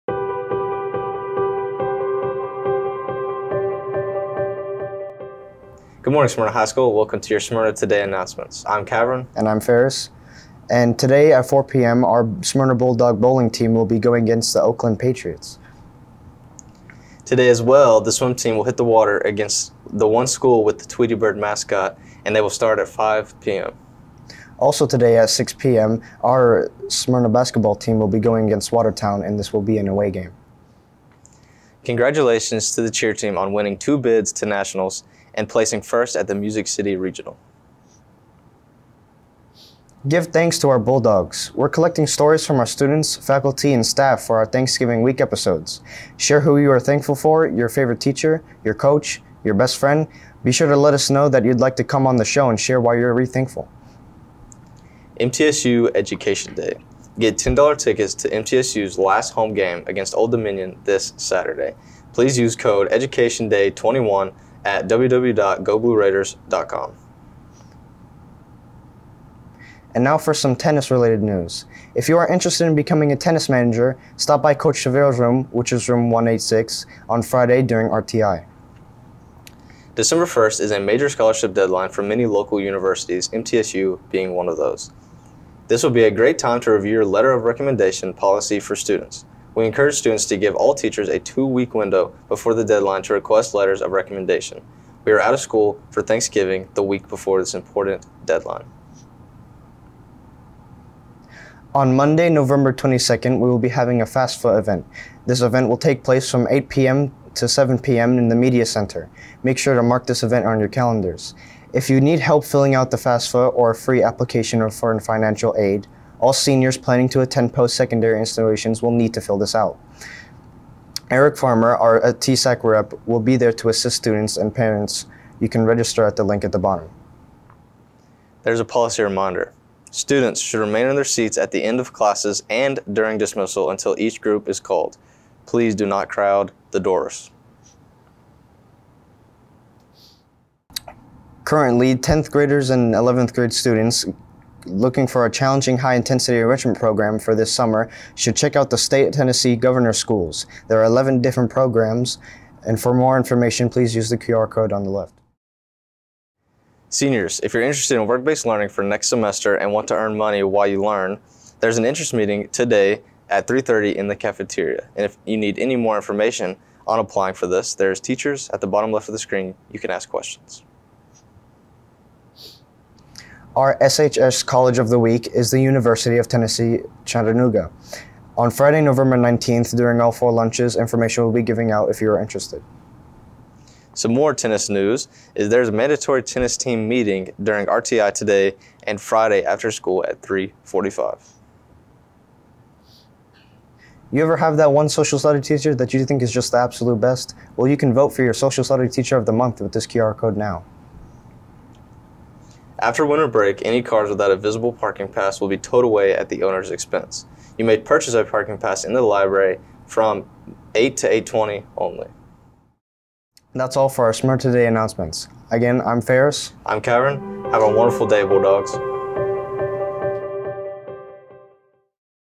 Smyrna Today - Announcements 11.18.21
The daily announcements for Smyrna high School
11.18.21

Director/Camera: Samuel Fisher
Asst Director: Emily Foster
Lighting Tech: Emily Foster
Audio Tech: Anna Lay